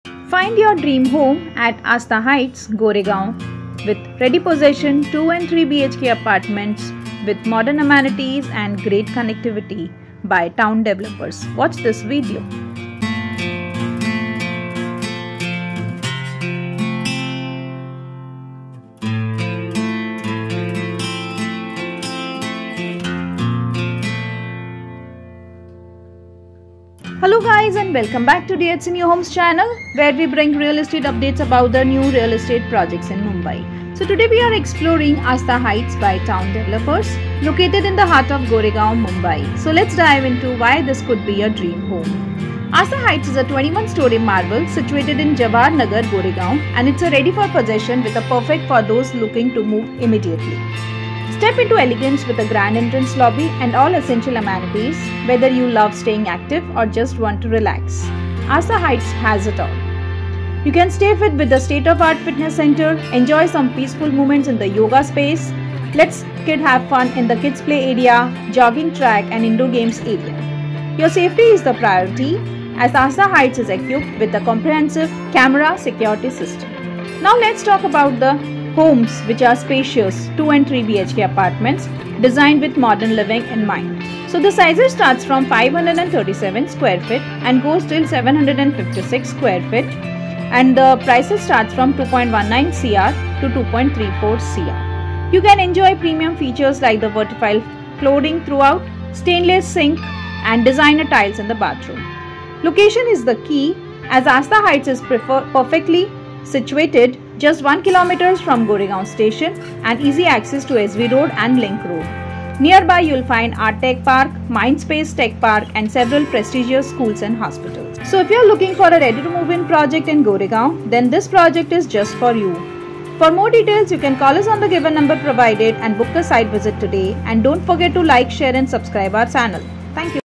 Ashtha Heights 📍Goregaon West, Mumbai | 2 BHK | 3BHK | flat in goregaon mumbai || Ready To Move In
ASHTHA HEIGHTS
📍Goregaon West, Mumbai

Name -  Ashtha Heights
Location - Goregaon, Mumbai
Developer - By Town Developers

Possession - Ready Possession Homes
Location - Jawahar Nagar, Goregaon

Project size - 106 Units

Products - 2, 3 BHK
Configurations for Town Ashtha Heights
2 BHK - 537 ft 2  -  ₹2.26 Cr
2 BHK - 610 ft 2 - ₹2.19 Cr
3 BHK - 756 ft 2 - ₹2.34 Cr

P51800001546
Registered with MahaRera